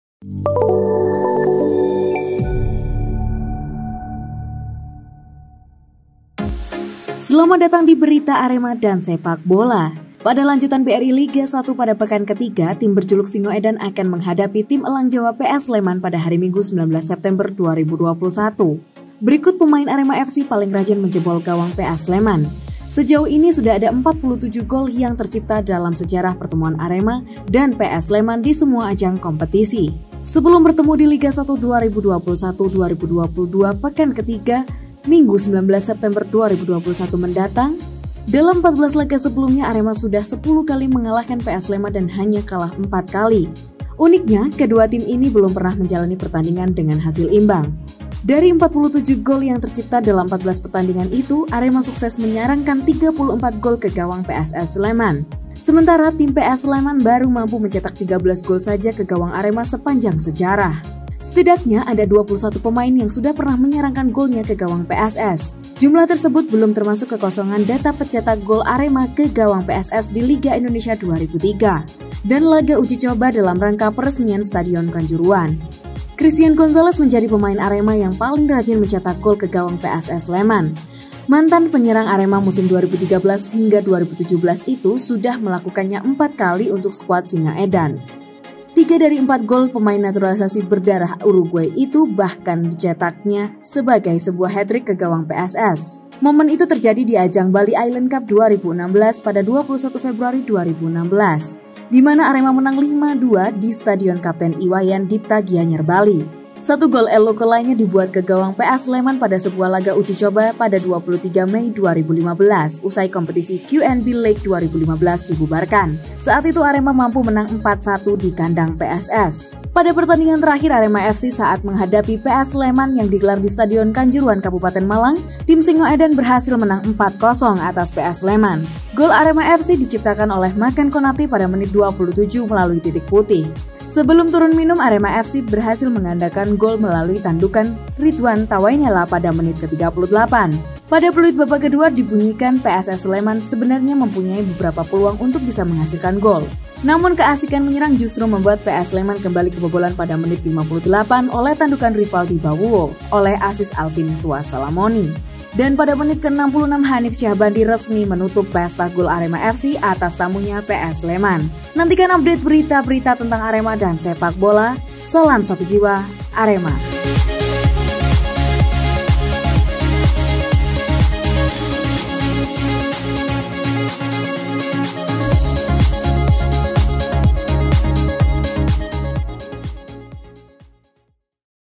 PEMAIN AREMA YANG RAJIN MENCETAK GOL DI GAWANG PSS SLEMAN | Liga 1 2021 - Wearemania
Dalam 14 laga sebelumnya, Arema sudah 10 kali mengalahkan PSS Sleman, dan cuma kalah empat kali. Kedua tim belum pernah menjalani pertandingan dengan hasil imbang.

Siapa saja penyumbang golnya?